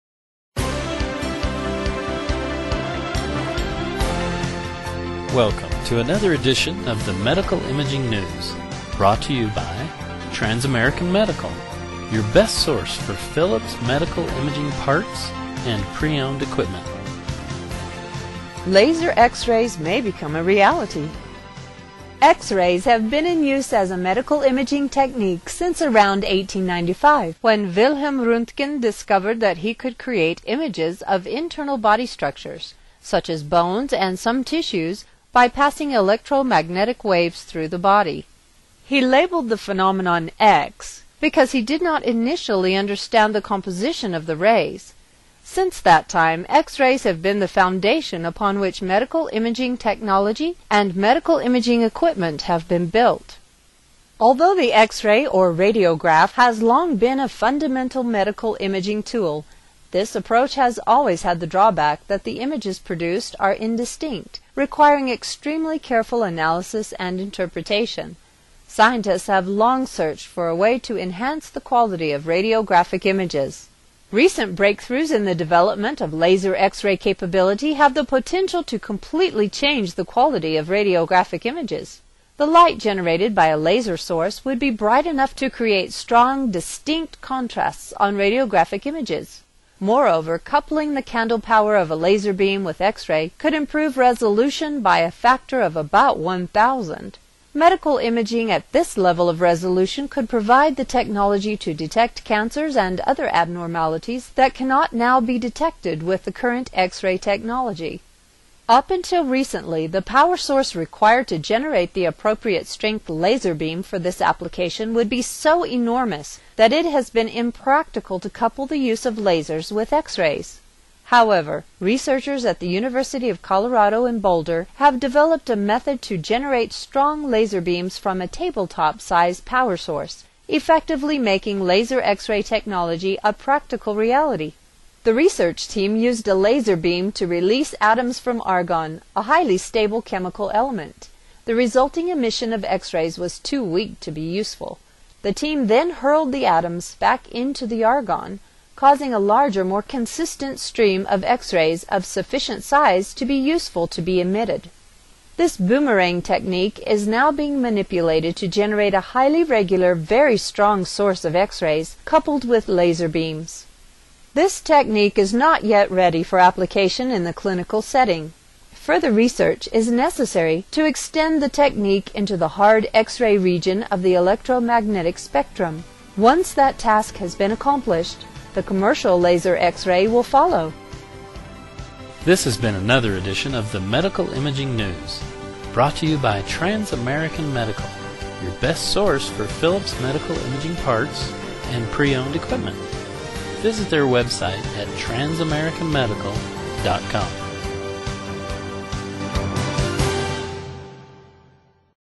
Medical Imaging News: Here Come Laser X-Rays
X-Rays have been in use as a medical imaging technique since approximately 1895 when Wilhelm Roentgen concluded that he was able to make photos of body structures like tissues and bones by passing electromagnetic waves through the body. He labeled the phenomenon "X" because he did not initially know the composition of the "rays".  X-Rays have been the building block upon which medical imaging technology and medical imaging equipment have been constructed.

Although the X-Ray has been a fundamental tool for medical imaging, this approach has always had the down side that the photos made are indistinct, which makes it so it requires very careful analysis and interpretation. Scientists have long searched for a way to enhance the quality of radiographic images.